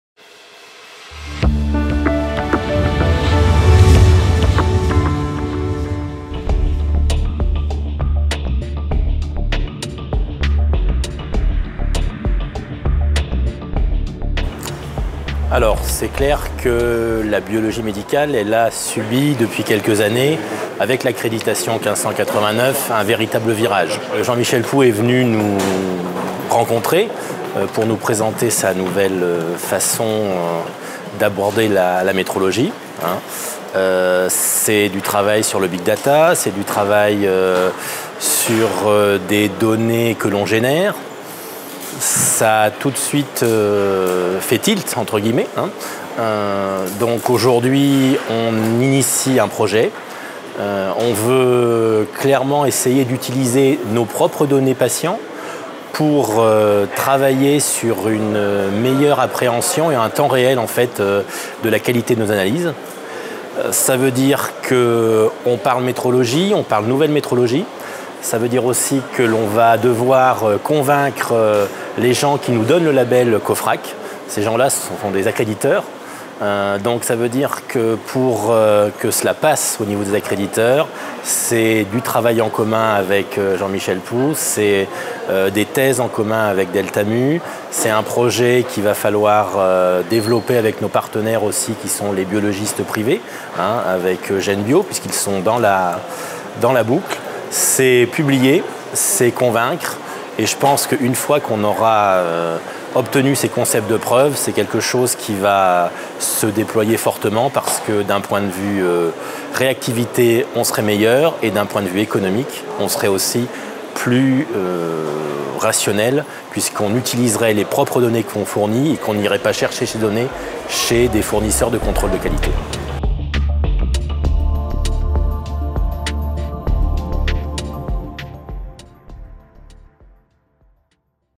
Smart Metrology - Interview 02 M. Sapin
M. Vincent Sapin, Responsable du pôle biologie médicale au CHU de Clermont-Ferrand, nous a fait l’honneur de participer à la journée du 9 Juin consacrée au lancement de la Smart Metrology. Écoutons-le nous expliquer pourquoi la Smart Metrology est une solution pertinente dans le domaine de la biologie médicale.